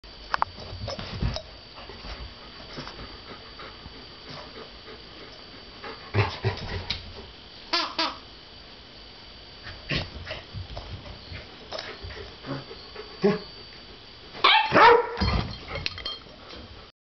Bark-off: Dewey and Mel
Dewey and Mel see whose bark is biggest.